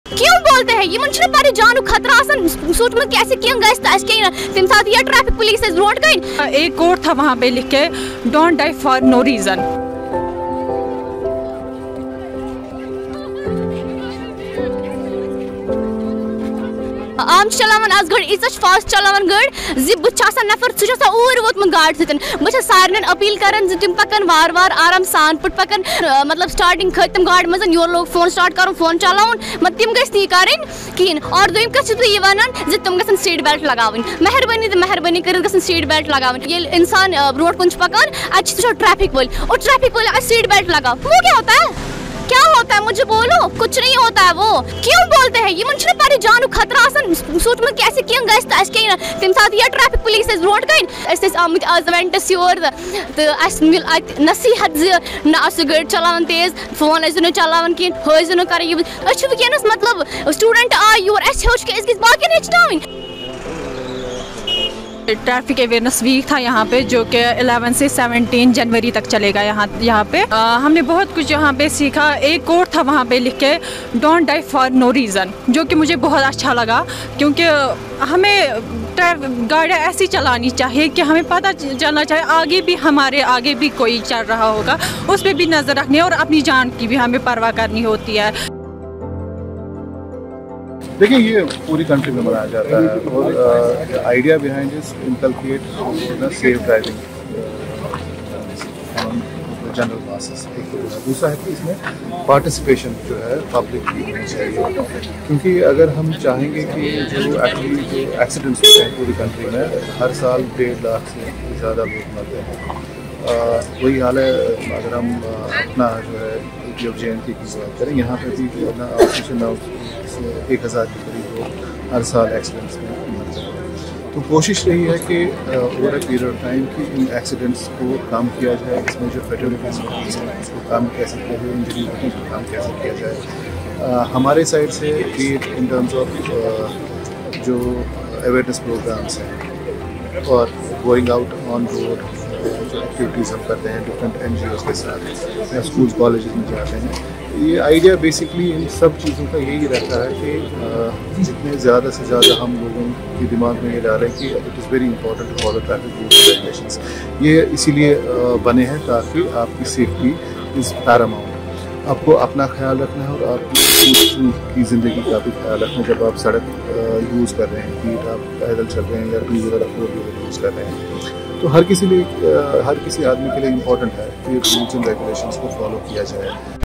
Week long National Road Safety Commences in Srinagar
Campaign aimed to educate citizens about the significance of road safetyVisit our